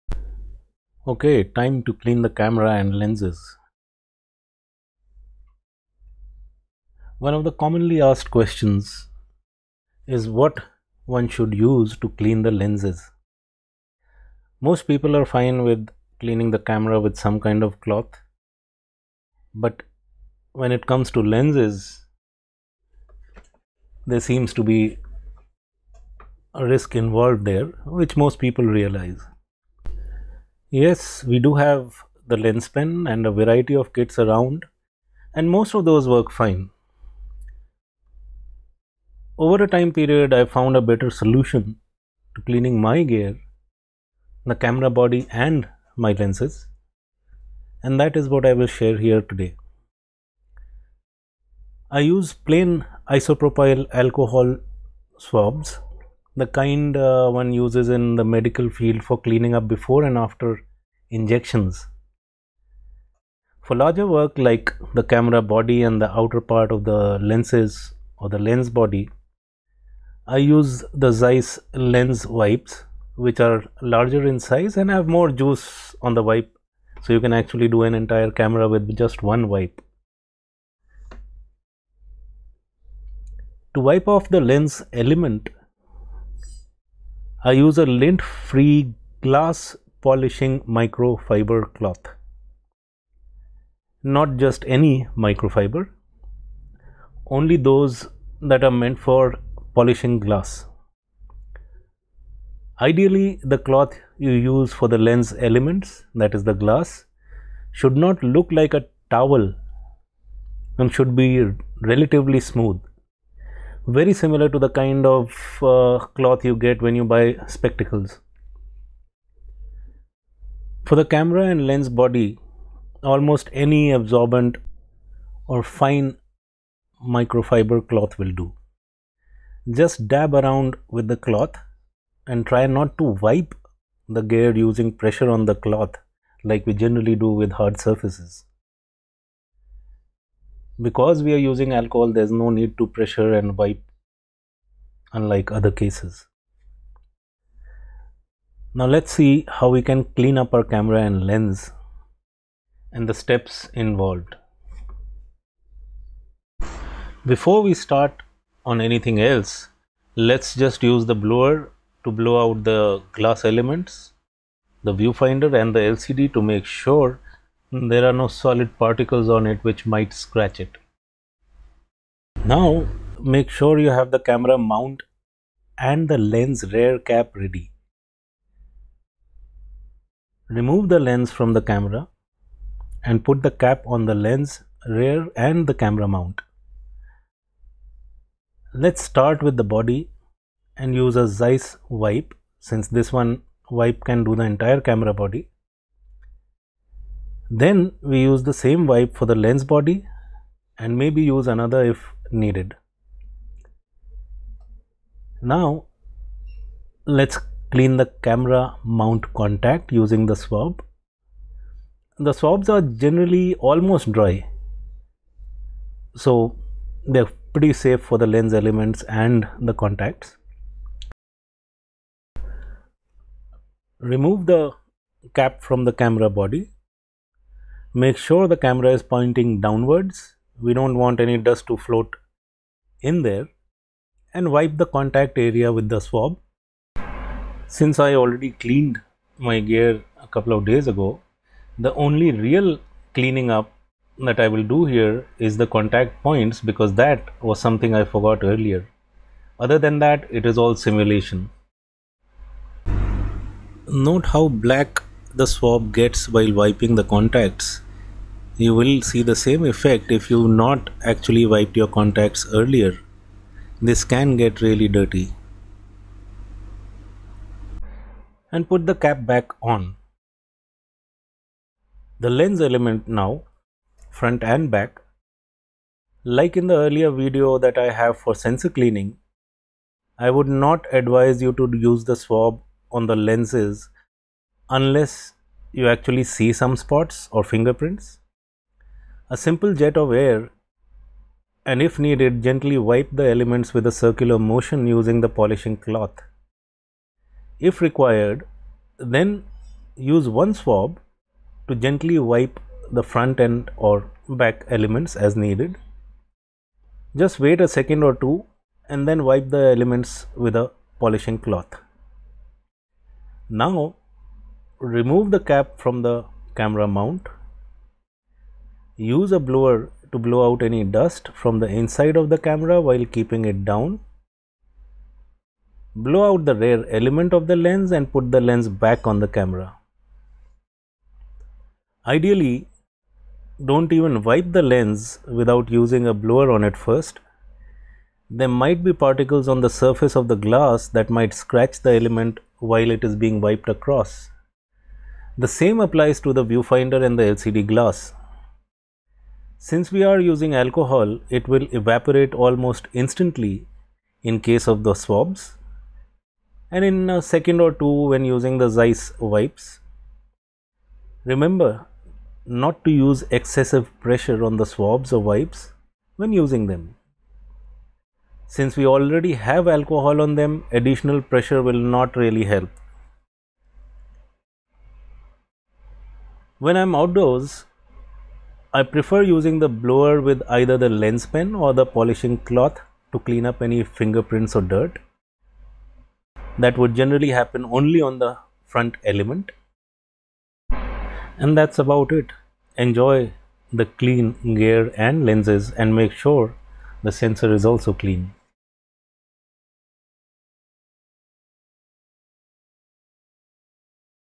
How to clean your camera and lenses
Time to clean the camera or lenses or both!

One of the commonly asked questions is what should I use to clean the lenses. Most people are fine with cleaning the camera with some kind of cloth, but, are wary when it comes to lenses.

Yes, we do have the lenspen and a variety of kits around and most of those work fine. Over a time period, I have found a better solution to cleaning my gear...Camera body and lenses.

I use plain isopropyl alcohol swabs, the kind we use in the medical field for cleaning up before and after injections. For larger work, like the camera body and the outer part of the lenses, I use Zeiss lens wipes which are larger in size and have more "juice" on the wipe.

To wipe off the lens element, I use a lint-free glass polishing micro-fibre cloth rather than any simple micro-fibre cloth. Ideally, the cloth you use which not look like a towel and should be smooth. Very similar to the kind you get when you buy spectacles.

For the camera and lens body, almost any absorbent, fine micro-fibre will do. Just dab around with the cloth and try not to wipe the gear using pressure on the cloth.

Let's see how we can clean up our camera and lens...

1. Make sure you have the camera mount and lens rear cap ready
2. Remove the lens from the camera and put the caps on the lens and the camera mount
3. Let's start with the body and use a Zeiss wipe since one wipe can do the entire camera body
4. We use the same wipe for the lens body or another if needed
5. Now, let's clean the camera mount contact using a swab. The swabs are generally almost dry. Remove the cap from the camera body. Make sure the camera is pointing down (we don't want any dust to float in) and wipe the contact area with the swab and put the cap back on
6. The lens elements now...Front and back. Like the in earlier video for sensor cleaning, I would not advise you to use the swab on the lenses unless you can see some spots or fingerprints. A simple jet of air and, if needed, just gently wipe the elements in a circular motion using the polishing cloth. If required, then, use one swab to gently wipe the front and/or back elements, wait for a few seconds and then wipe the elements with the polishing cloth.
7. Now, remove the cap from the camera mount. Use the blower to blow out any dust from the inside of the camera while keeping in down. Blow out the rear element of the lens and put the lens back on the camera.

Ideally, don't even wipe the lens without using a blower on it first. There might be particles on the surface of the glass that might scratch the element while being wiped across. The same applies to the viewfinder and LCD glass.

Since we are using alcohol, it will evaporate almost instantly in the case of the swabs and in a few seconds when using the Zeiss wipes. Remember not to use excessive pressure on the swabs or wipes when using then. Since we already have alcohol on them, additional pressure will not do anything.

When I am outdoors, I prefer using the blower with either the lenspen or the polishing cloth to clean up any fingerprints or dirt that might have fallen on the front element.

Since I did not clean the contact areas, that is all that I will actually clean here...The rest is "understood" types :)

That's it!

Done.